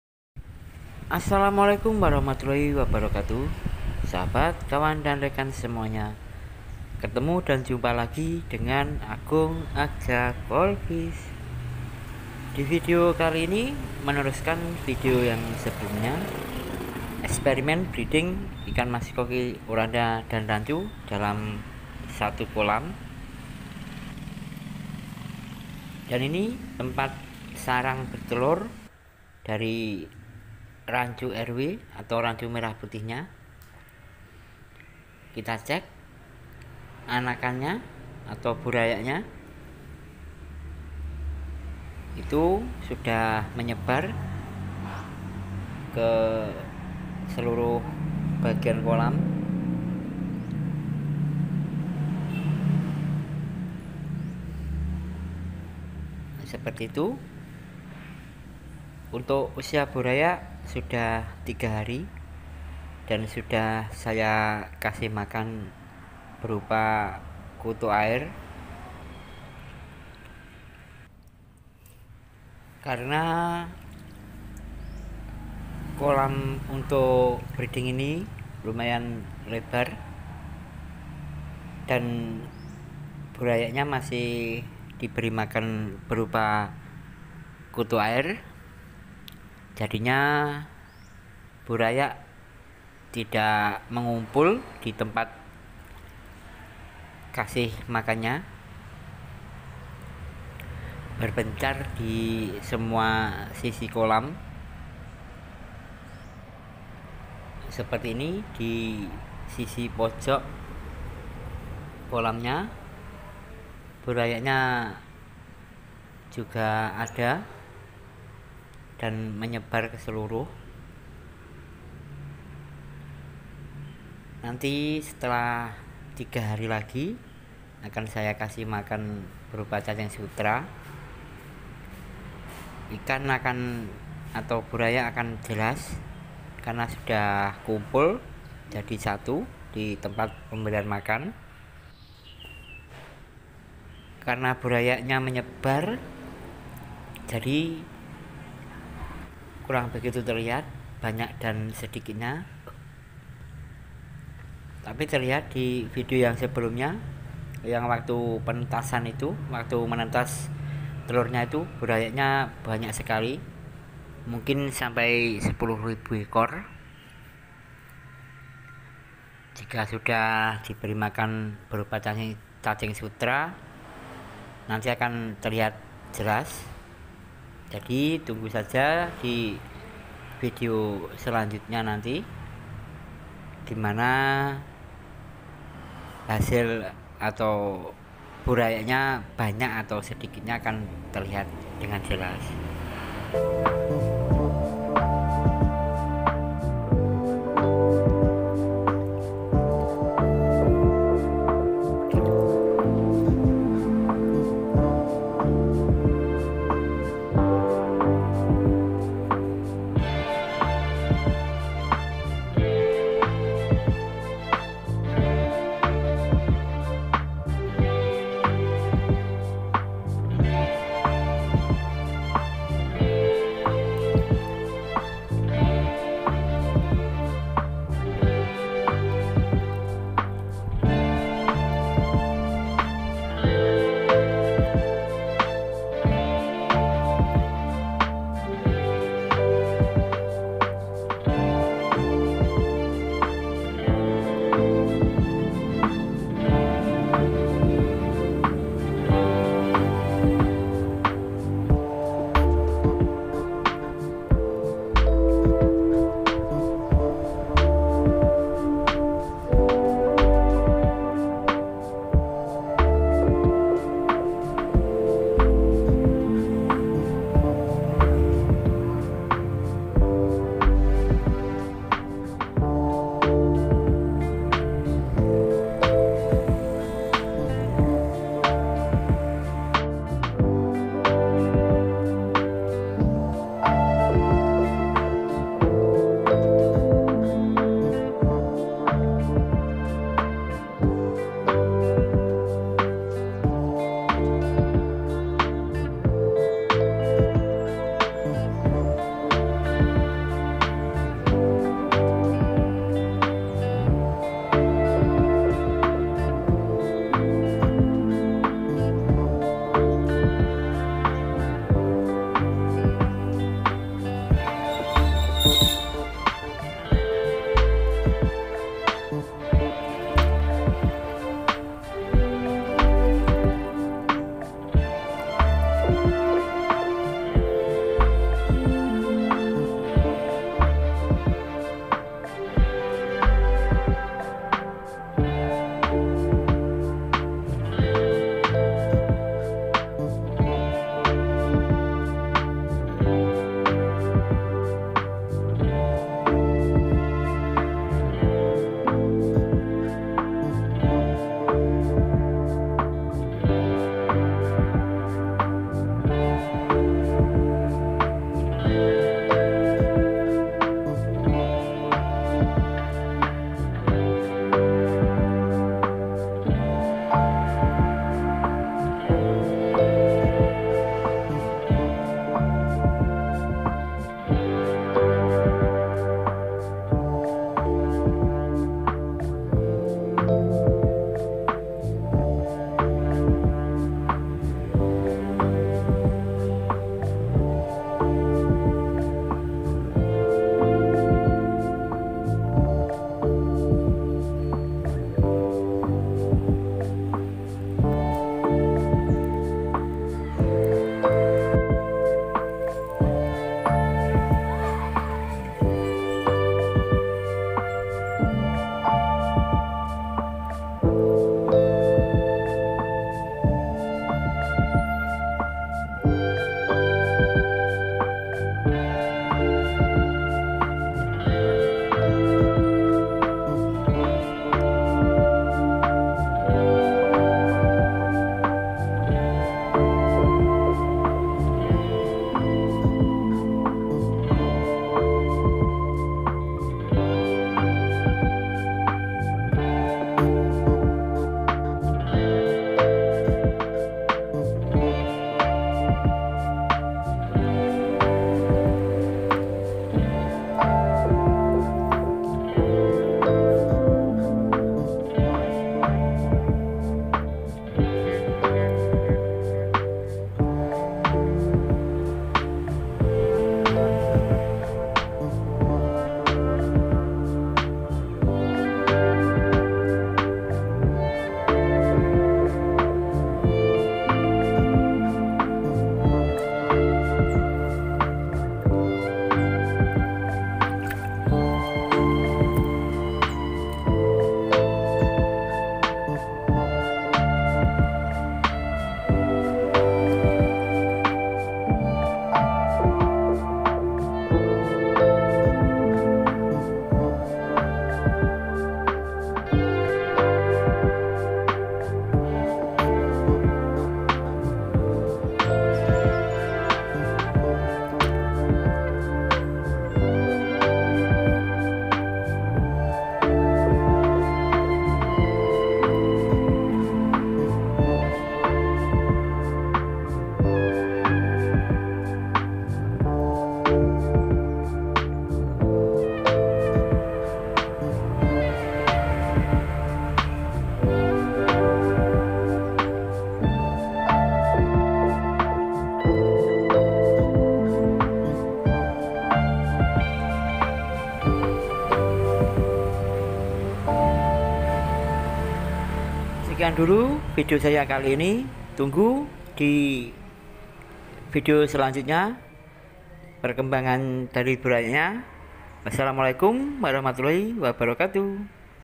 Burayak Usia 3 Hari, Hasil Experimen Breeding Ikan Maskoki Oranda dan Ranchu dalam satu kolam
Assalamualaikum sahabat kawan dan Saudara semuanya jumpa lagi dengan Agung Agja Goldfish
di video kali ini melanjutkan video sebelum, tentang Experimen Breeding ikan maskoki Oranda dan Ranchu dalam satu kolam

Wassalamu'alaikum